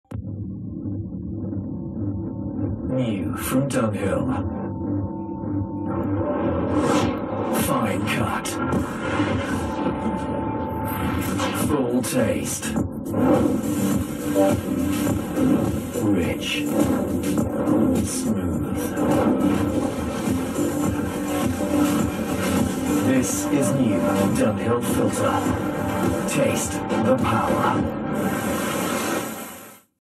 Iklan Dunhill Filter Black - In A New Perspective (2014) @ Trans TV, Indosiar, RCTI, & SCTV
Iklan ini pernah ditayangkan di Trans TV, Indosiar, RCTI, & SCTV pada tahun 2014.
Sumber: @amnesianida